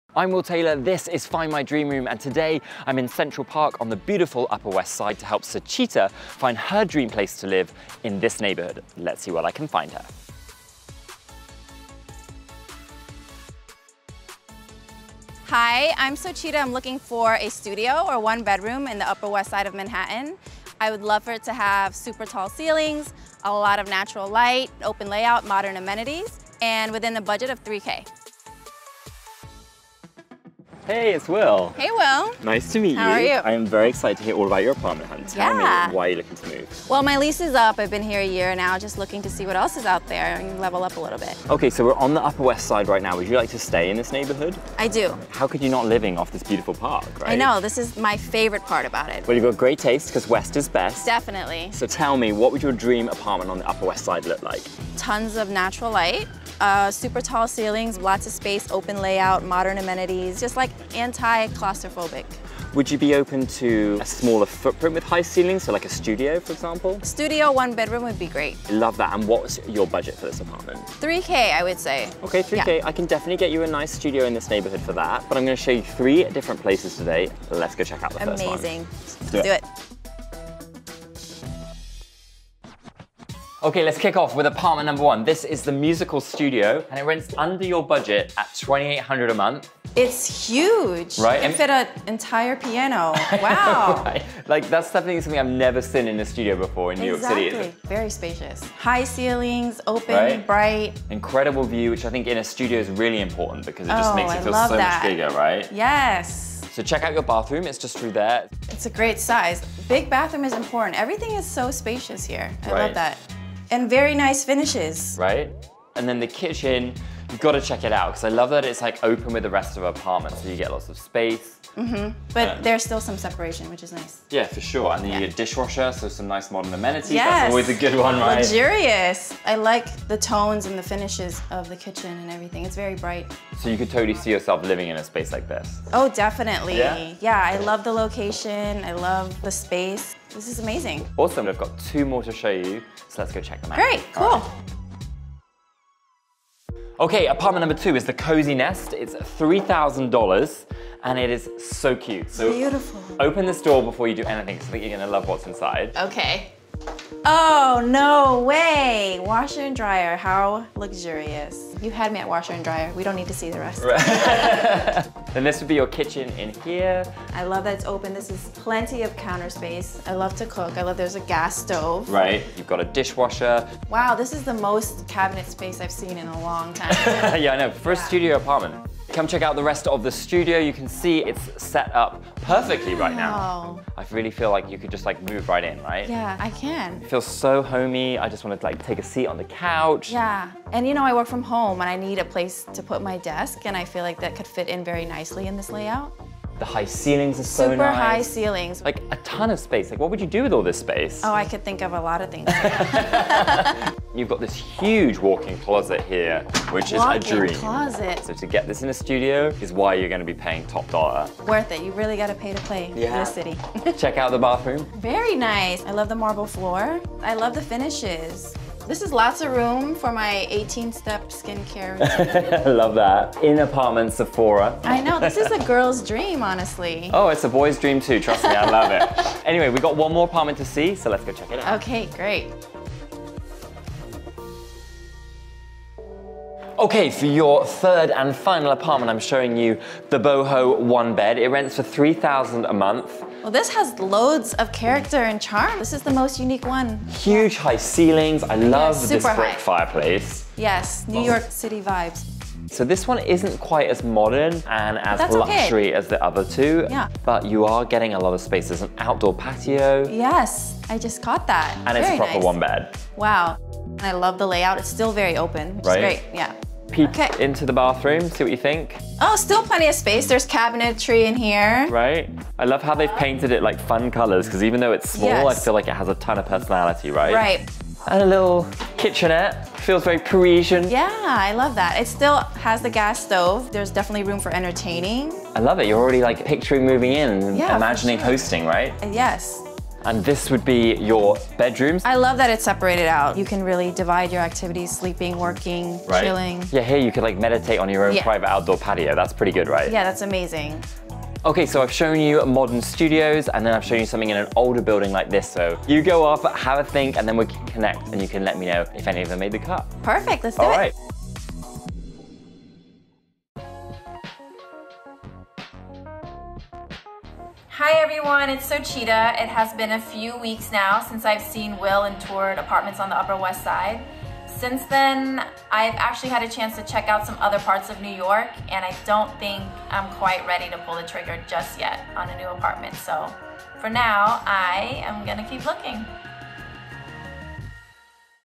New Yorker Socheata is apartment hunting for a space with high ceilings and plenty of natural light
Host Will Taylor is taking Socheata on a tour of three potential apartments in the Upper West Side neighborhood of Manhattan in hopes that she can find her dream home.